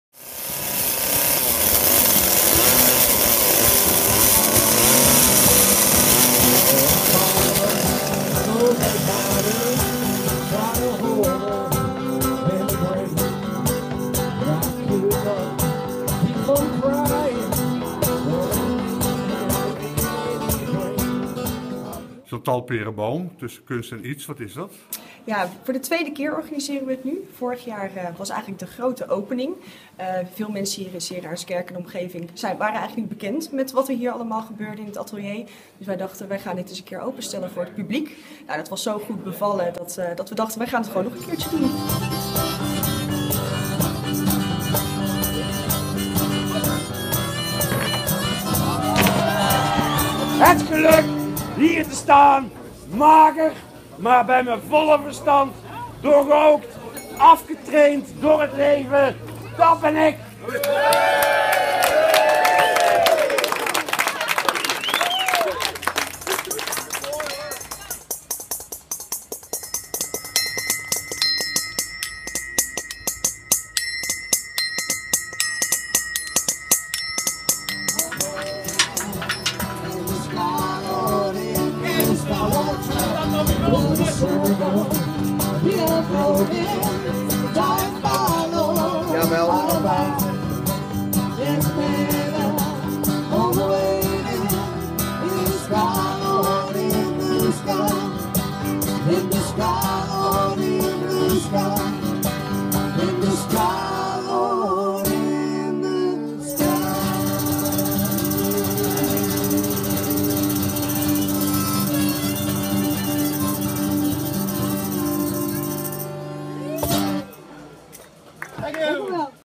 TUSSEN KUNST & IETS
Voor de tweede keer organiseert Atelier het Raadhuis (Arendskerke) Tussen Kunst & Iets. Een vrolijke open dag met veel kunst, muziek, bellen en toeters.